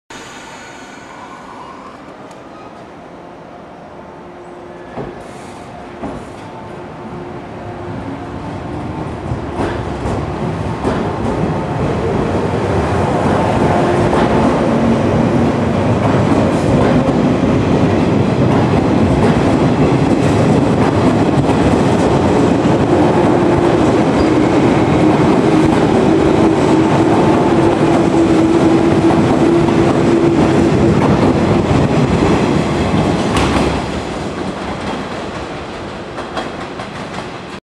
113系マリ117編成+マリS62編成 千葉駅発車
撮影地：千葉駅
小湊鉄道に行く際にたまたま湘南色10連に出会えたので撮影しておきました。今はマリ117編成のクハが夢ハウスあずさにありますので行ってみてはいかがでしょうか？